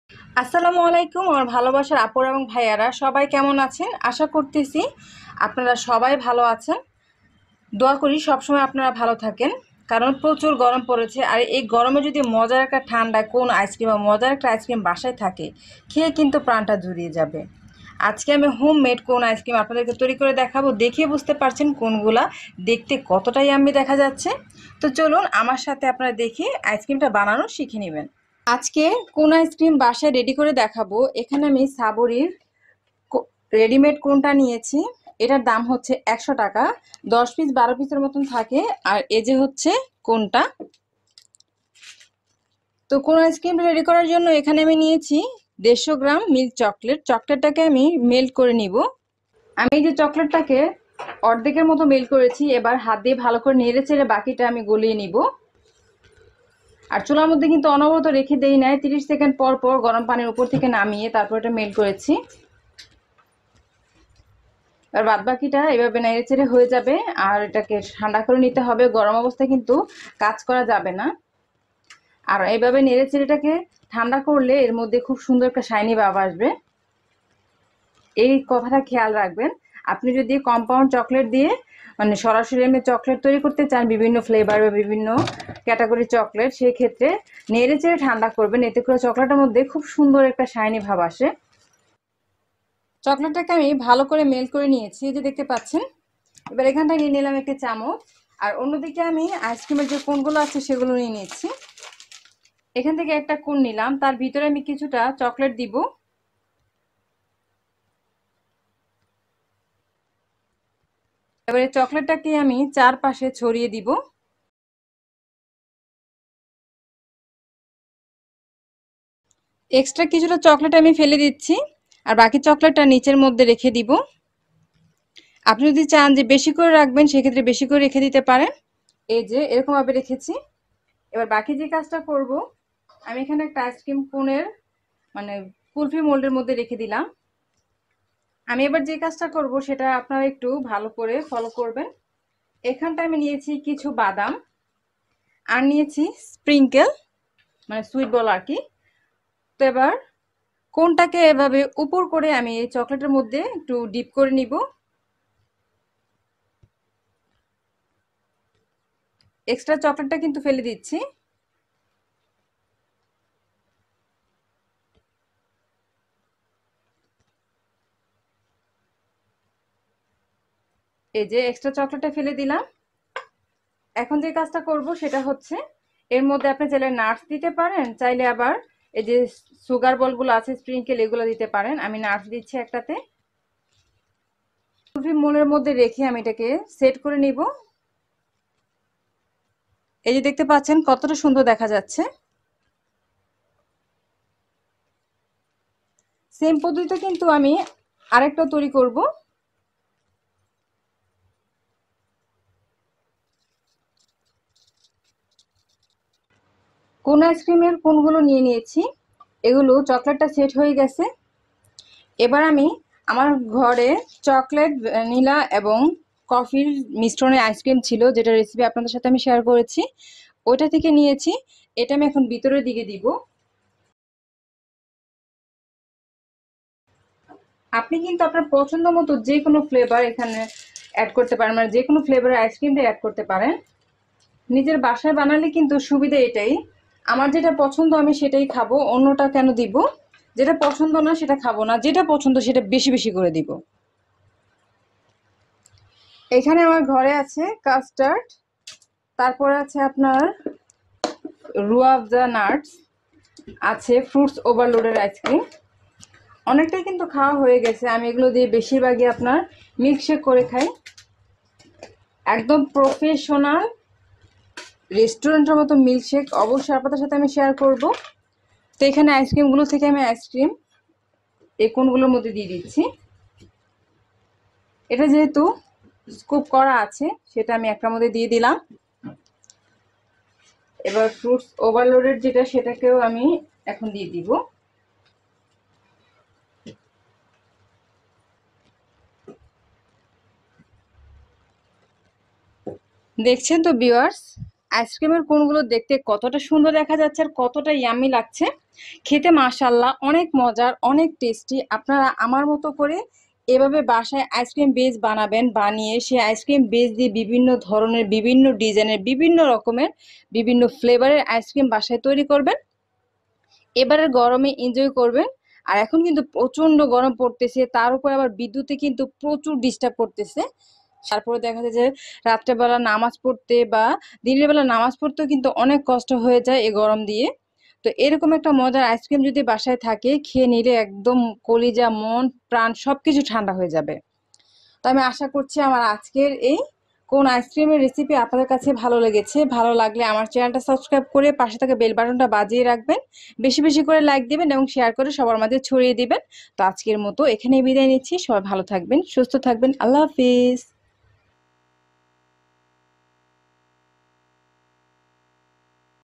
দোকান থেকে কোণ আইসক্রিম না কিনে ঘরে তৈরি করুন কোণ আইসক্রিম | Homemade cone ice cream
Hello,My friends,
Support my Hobbi's Cook channel.I always try to making easy recipe.
Yummy and healthy recipes share.Thanks for all friends.